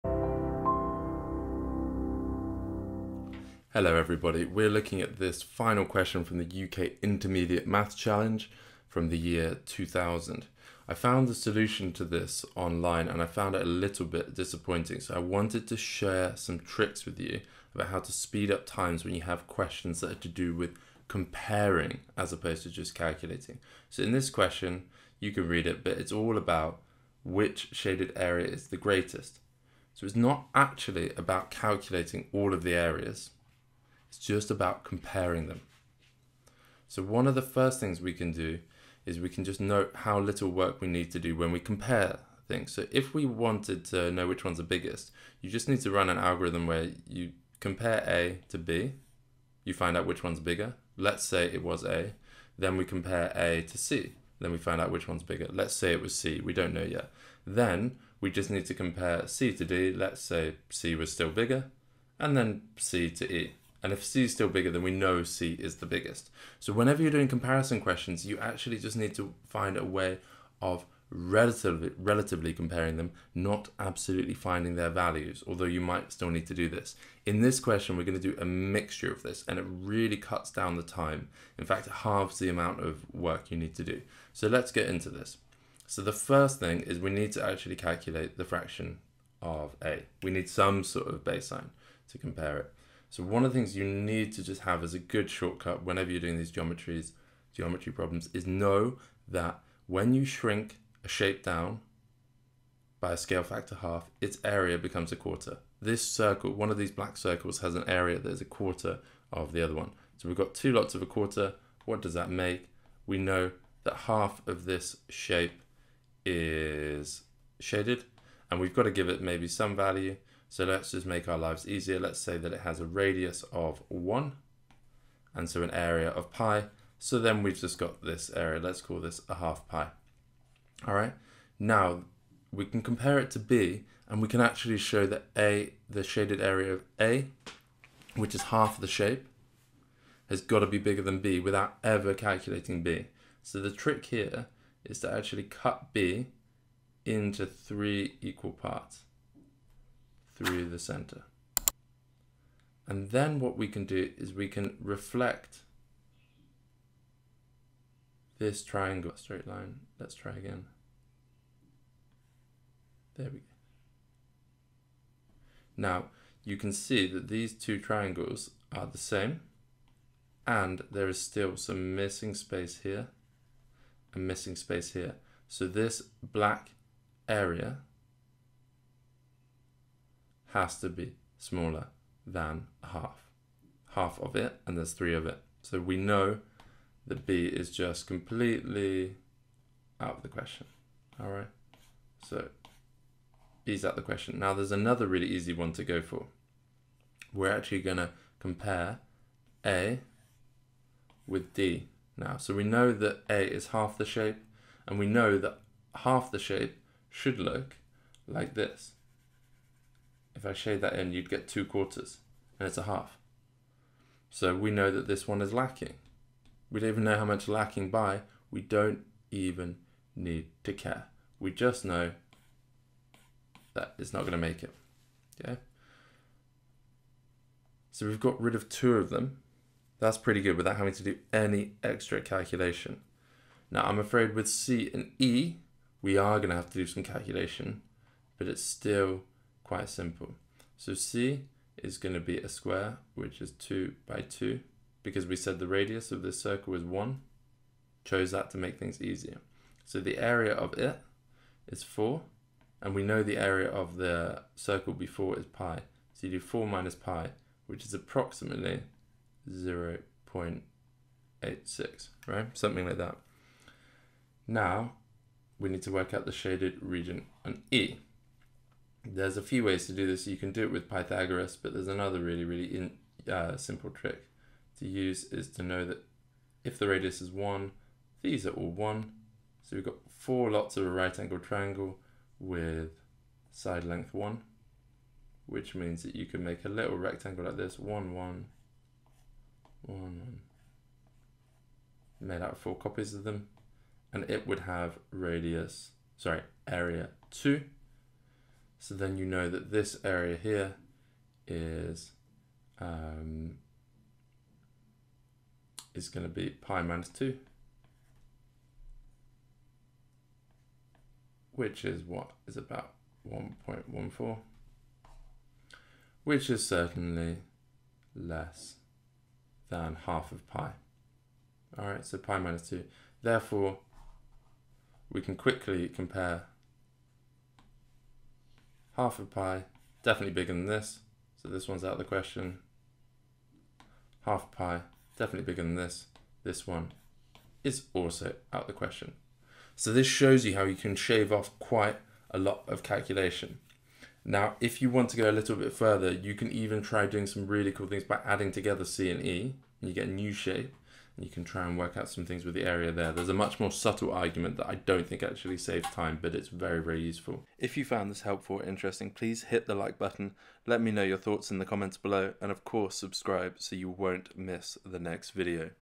UK Maths Challenge | Shortcuts For Finding Greatest or Least
This is a great problem from Q25 of the UK Intermediate Maths Challenge 2000 that the official solution to doesn't capture a very neat time-saving trick.

When comparing, you don't always need to work out the absolute value of each, only to show that one exceeds the other. This is similar to how we tackle problems in the real world, problems that usually call for choosing between constrained options rather than calculating in the abstract.